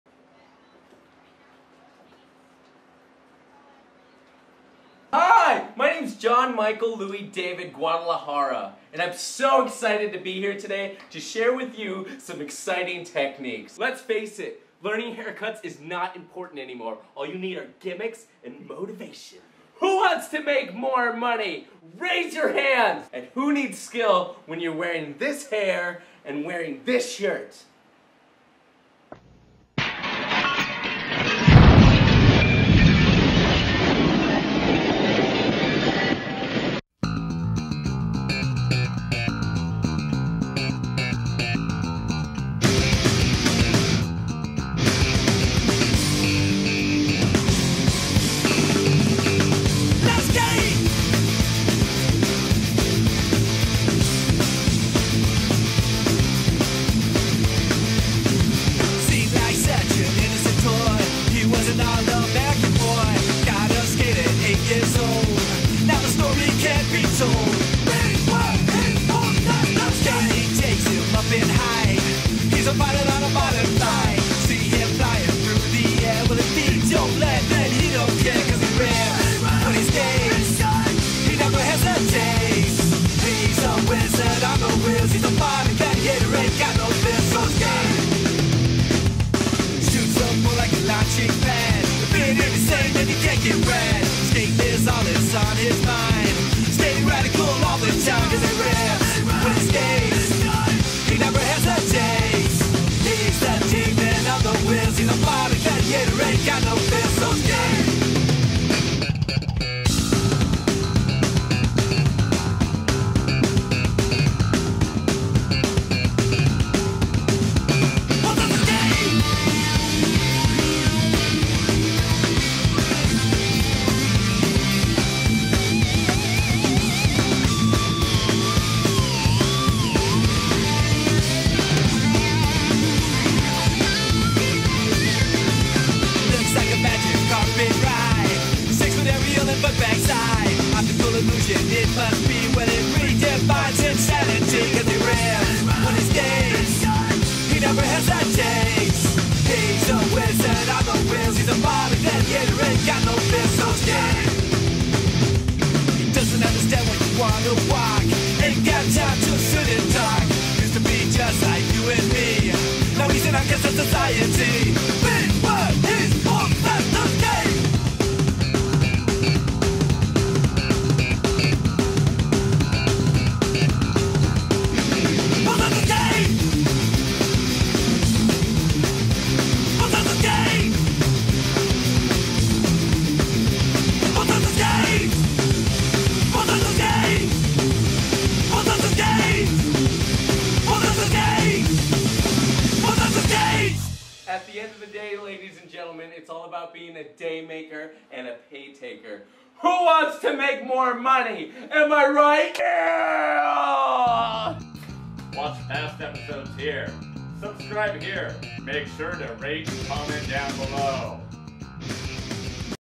JoshXO Presents Short Attention Span Haircuts. THRASHIN
Who's got time for full length videos? Get all the important parts of this "80's Skate Video" cut in less than 5 min.
JoshXO's new direction in hair videos will bring humor and interesting themes to break the classic boring mold of hairdressing videos.